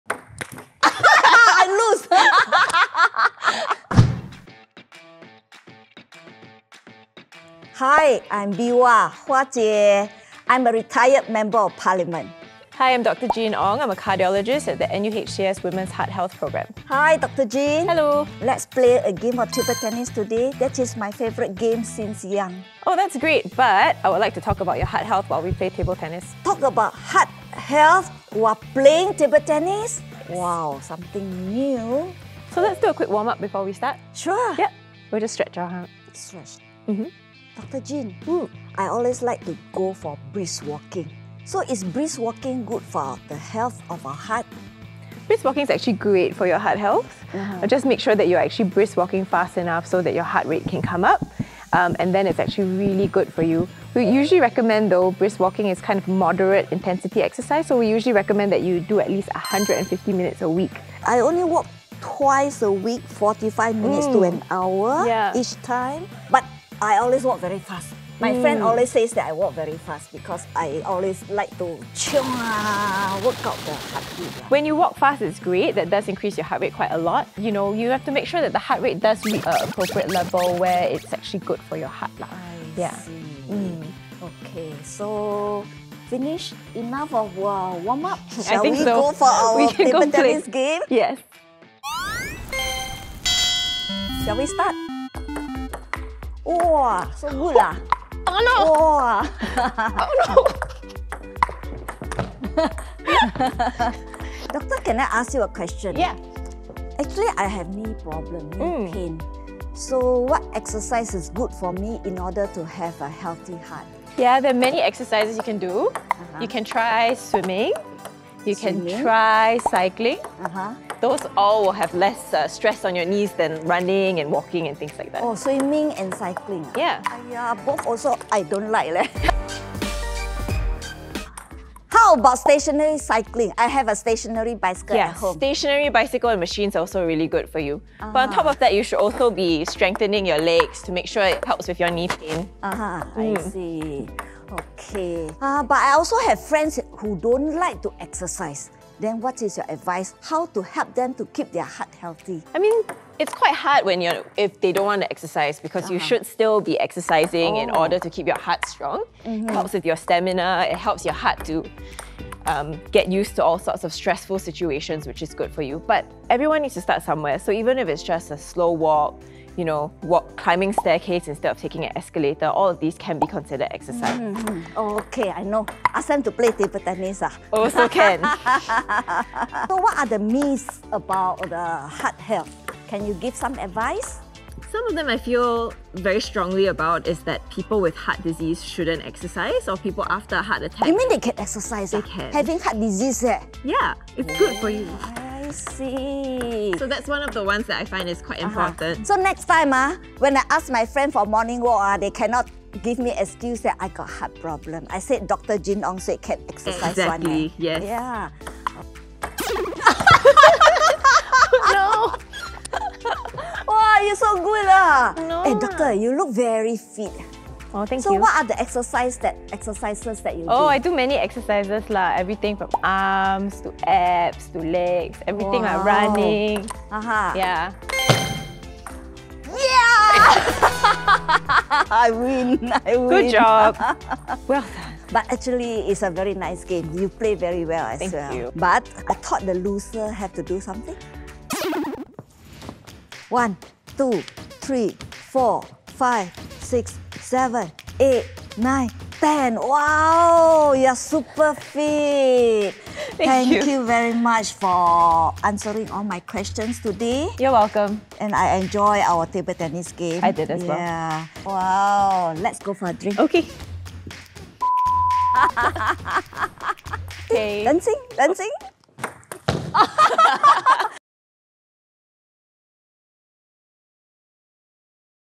It's 'ping pong' time! 🏓
Catch Lee Bee Wah (Hua Jie) and NUHCS Heart Expert, Dr. Jeanne Ong in this workout as they “serve” important questions on exercise 🚴 and heart health. Stay active to lower your risk of cardiovascular disease! 💖